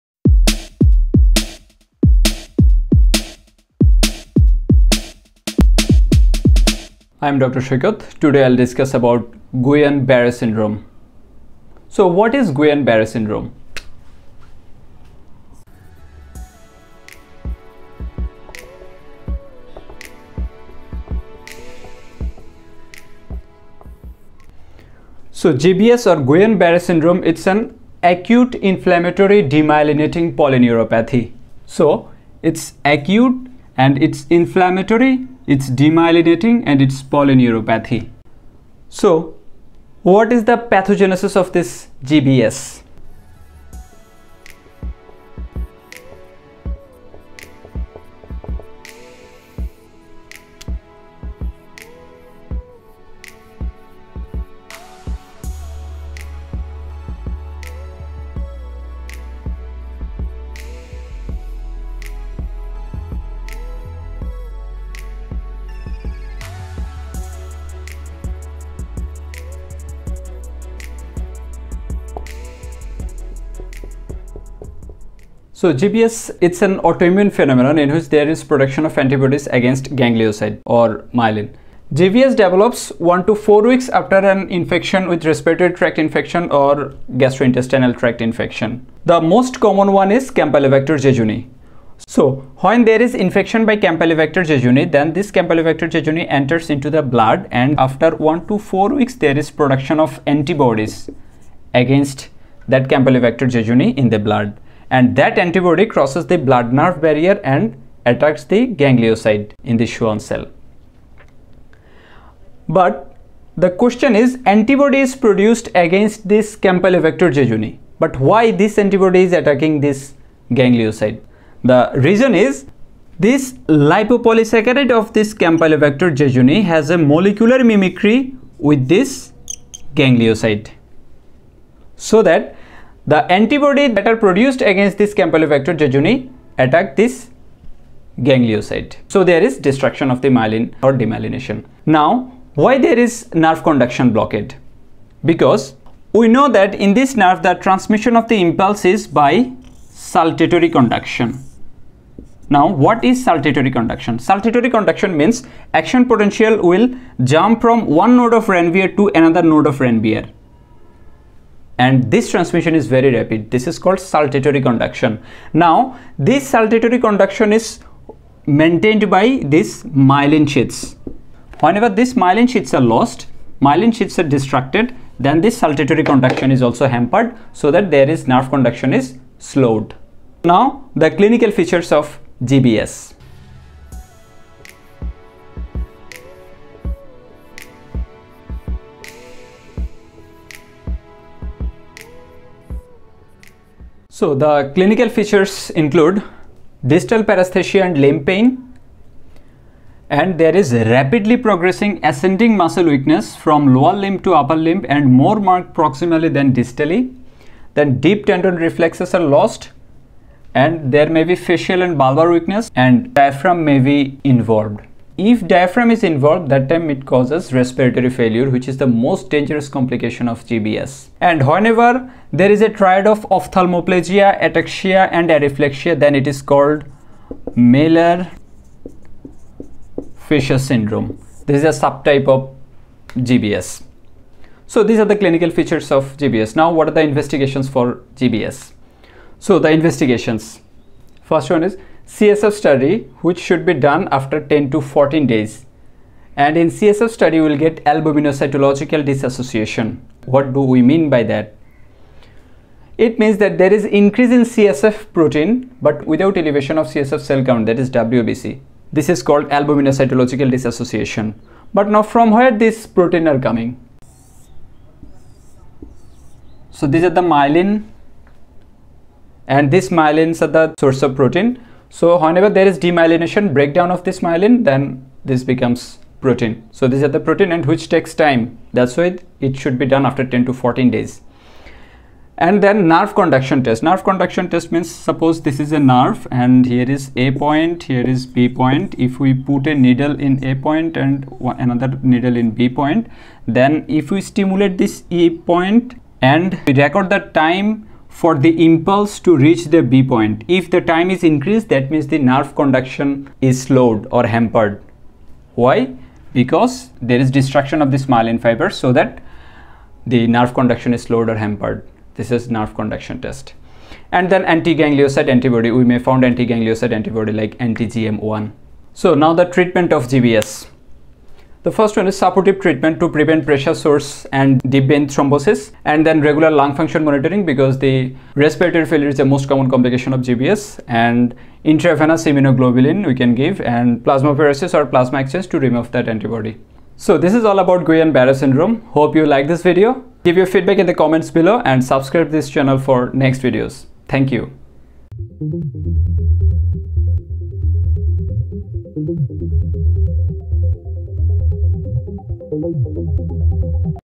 Guillain-Barre Syndrome (GBS) | Pathogenesis, C/F, Investigations & Treatment | Dr. Saykat
The basic concepts of Guillain-Barre Syndrome (GBS) has been discussed in this video. Pathogenesis, Clinical Features, Investigations, basics of Nerve conduction test & Treatment options are also discussed in brief.

- Dr. Saheduzzaman Bhuiyan Saykat